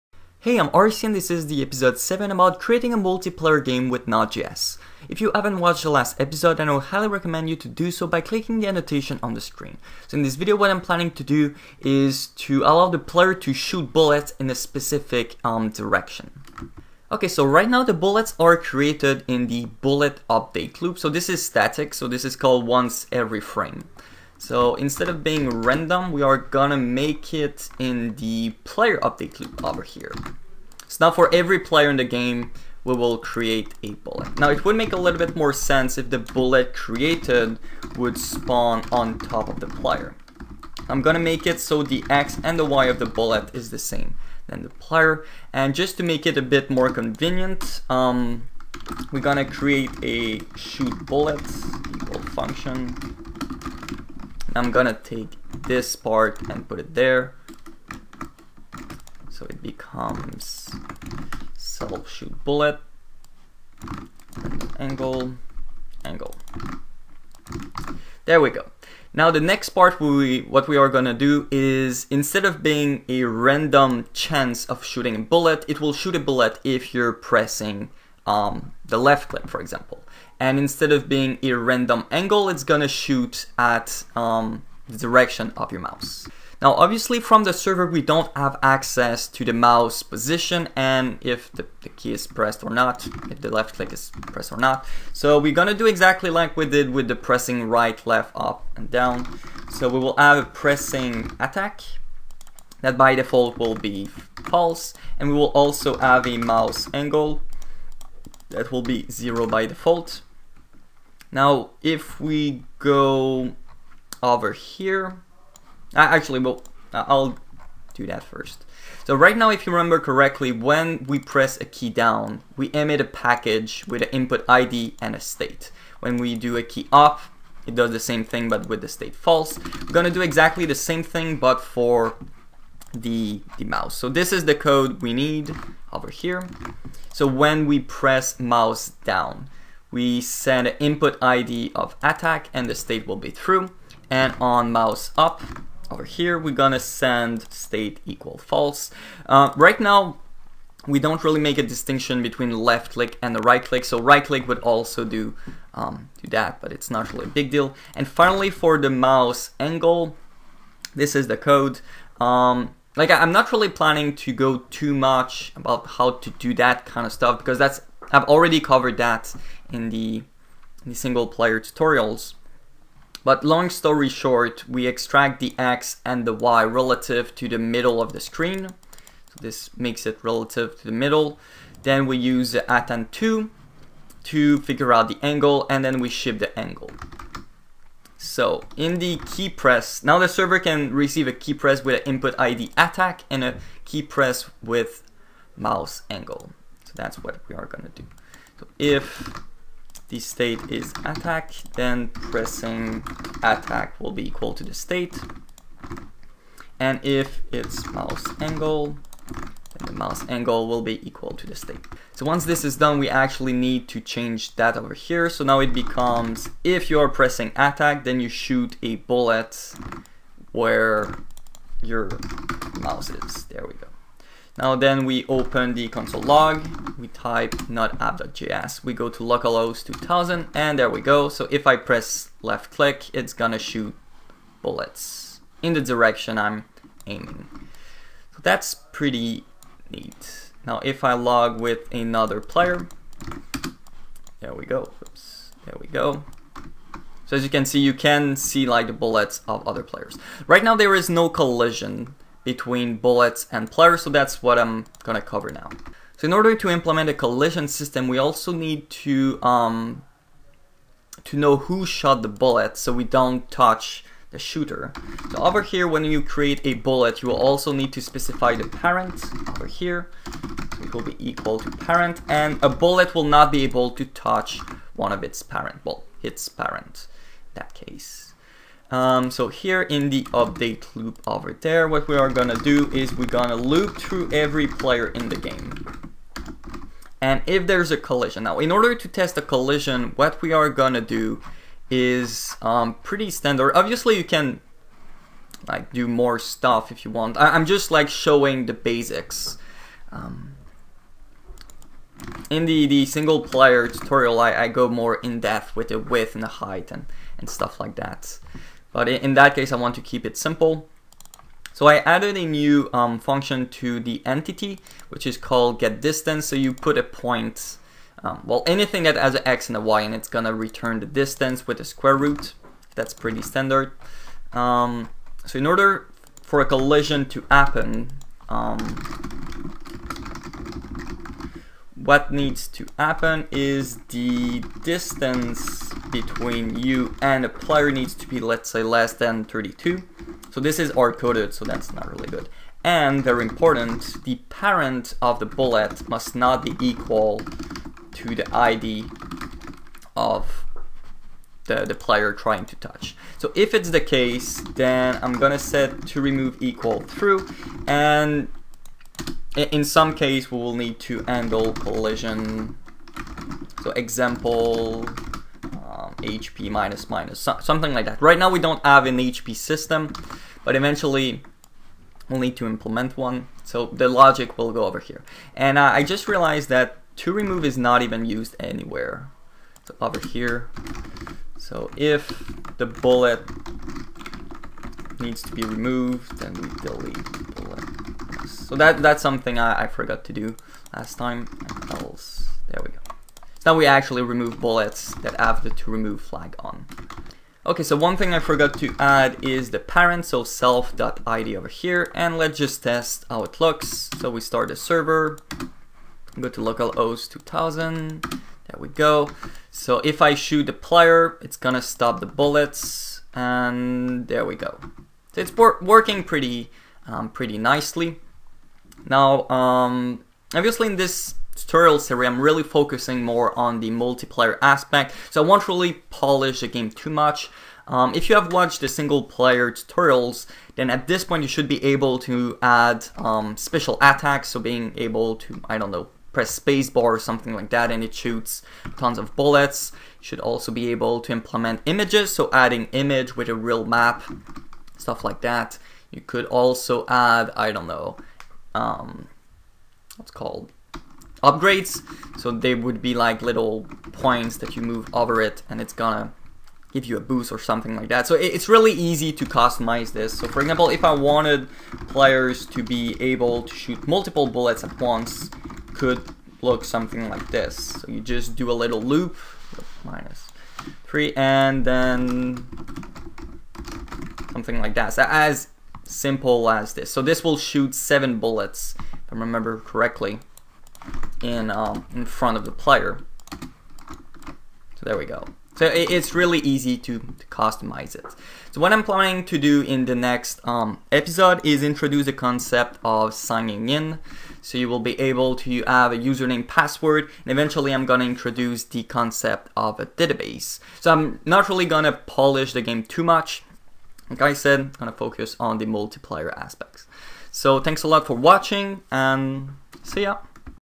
7- Making Multiplayer HTML5 Game: Player Attacks. NodeJs Tutorial Guide
Episode 7 about making a multiplayer video game in HTML5 using Nodejs. In this video, I cover how to make the player shoot bullets with his mouse.